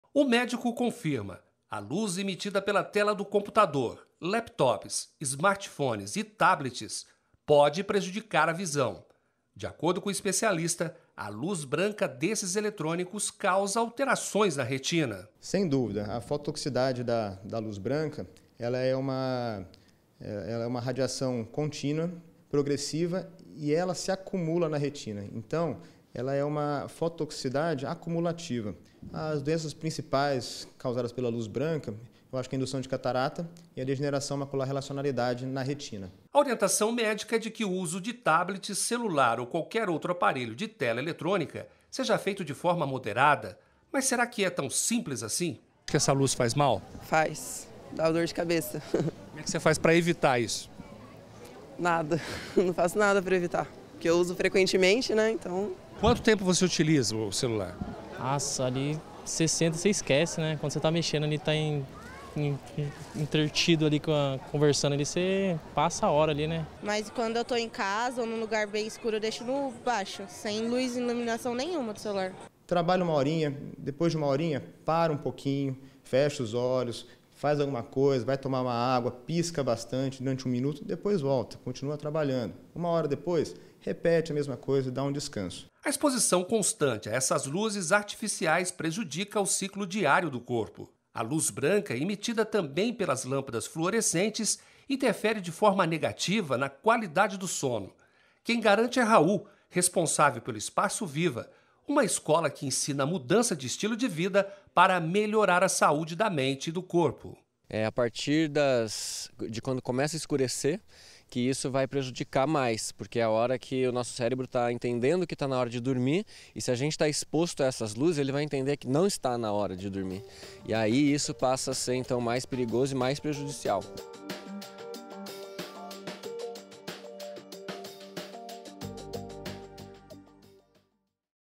LUZ BRANCA, SEUS EFEITOS NA SAÚDE
Reportagem produzida pelo jornalismo da Tv Novo Tempo